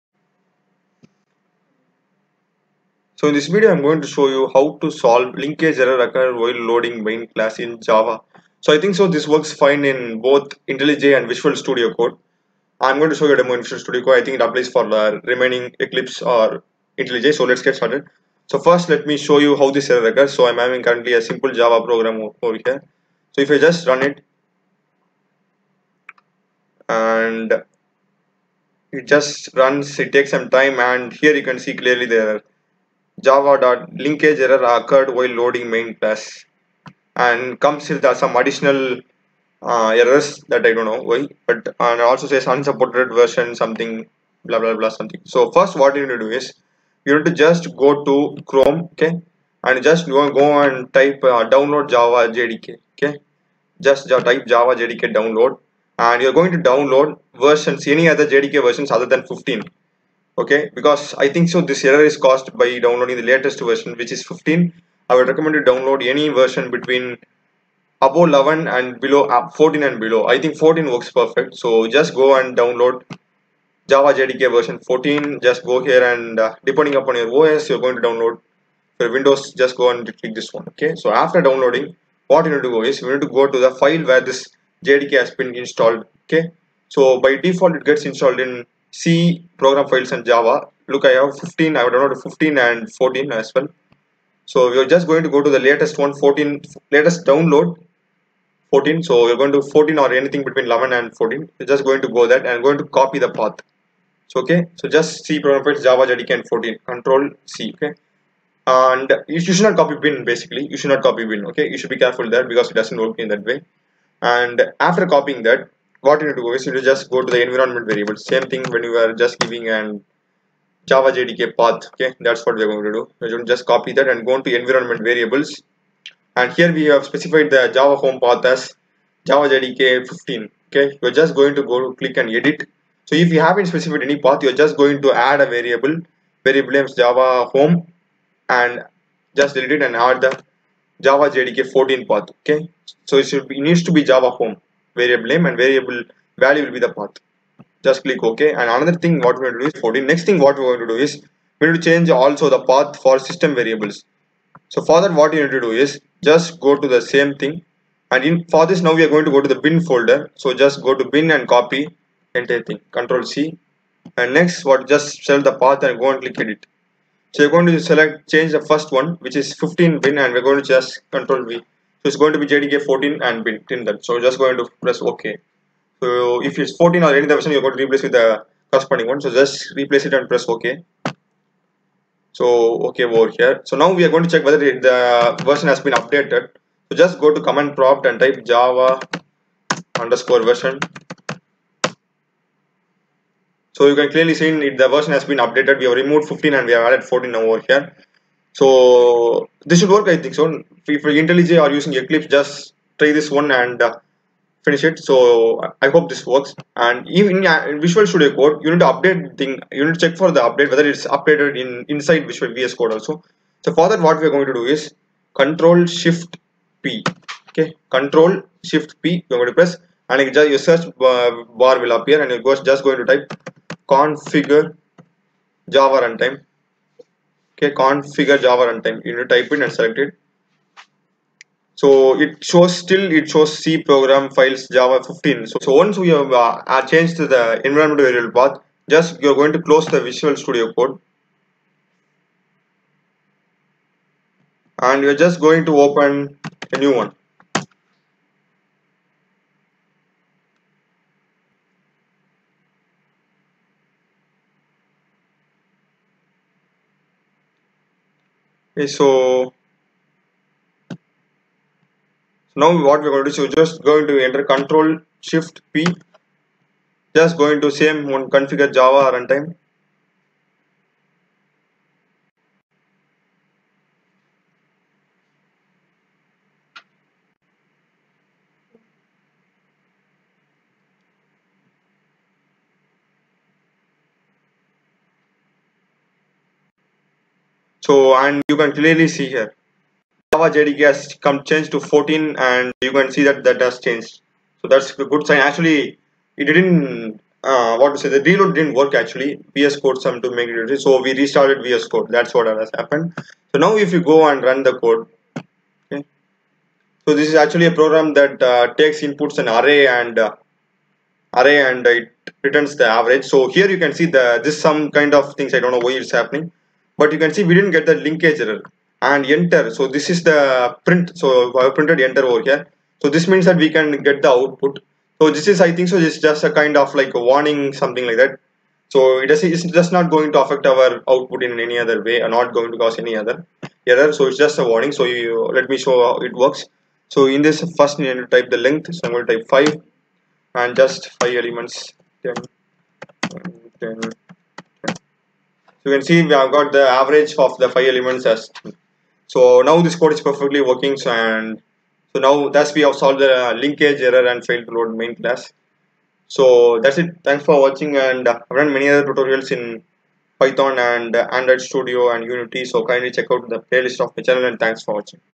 Linkage error occured while loading main class java in Visual Studio code | Eclipse | Intellij
Linkage error occured while loading main class java in Visual Studio code ,Eclipse, Intellij solved
java.lang.unsupported class version error
'chcp.com' is not recognized as an internal or external command